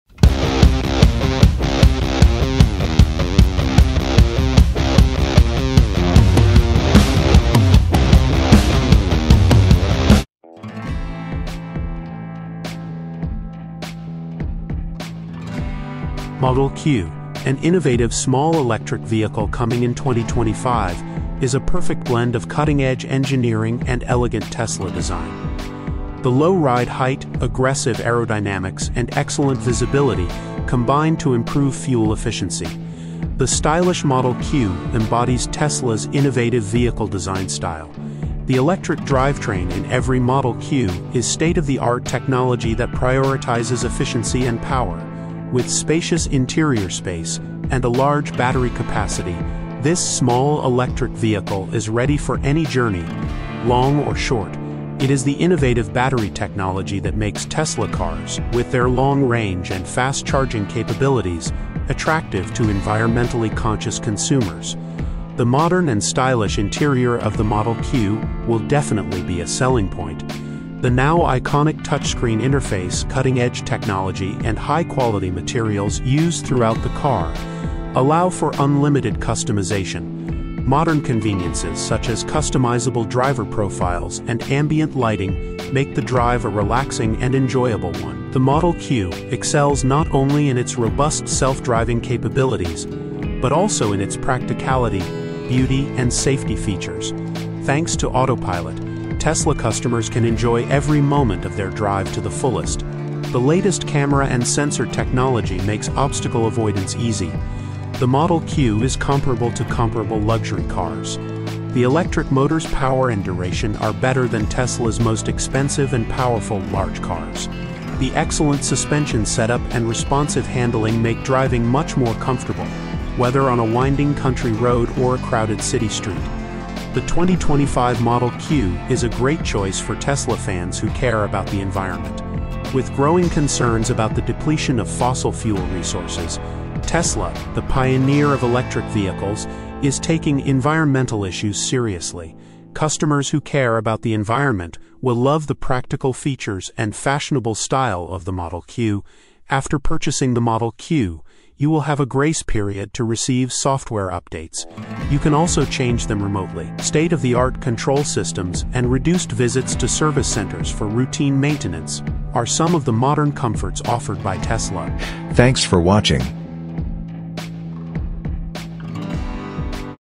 A New 2025 Tesla Model Q Unveiled - Revolutionizing The Technology Of Electric SUVs !
Model Q, an innovative small electric vehicle coming in 2025, is a perfect blend of cutting-edge engineering and elegant Tesla design. The low ride height, aggressive aerodynamics, and excellent visibility combine to improve fuel efficiency. The stylish Model Q embodies Tesla's innovative vehicle design style.

The electric drivetrain in every Model Q is state-of-the-art technology that prioritizes efficiency and power. With spacious interior space and a large battery capacity, this small electric vehicle is ready for any journey, long or short. It is the innovative battery technology that makes Tesla cars, with their long range and fast charging capabilities, attractive to environmentally conscious consumers.

The modern and stylish interior of the Model Q will definitely be a selling point. The now iconic touchscreen interface, cutting-edge technology, and high-quality materials used throughout the car allow for unlimited customization. Modern conveniences such as customizable driver profiles and ambient lighting make the drive a relaxing and enjoyable one.